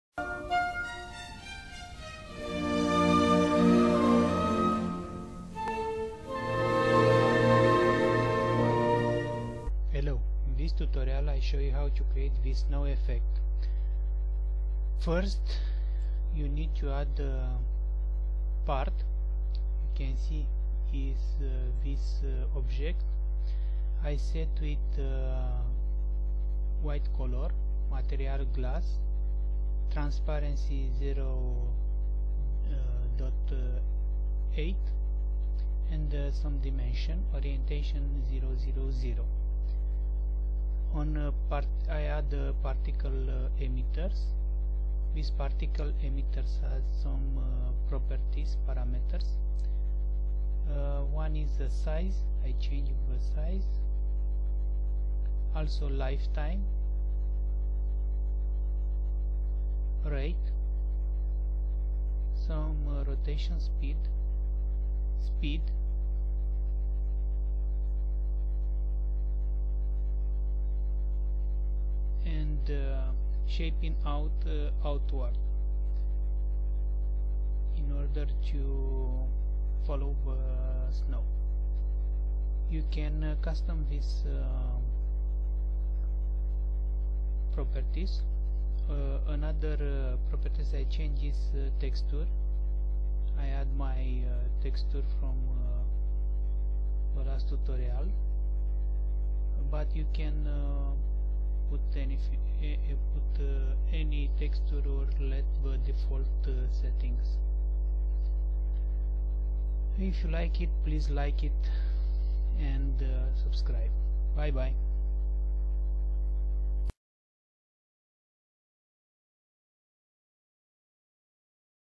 Roblox snow effect .
In this video tutorial I will show you how to make a snow effect in Roblox.
It is not the most complete example but at first it is quite good.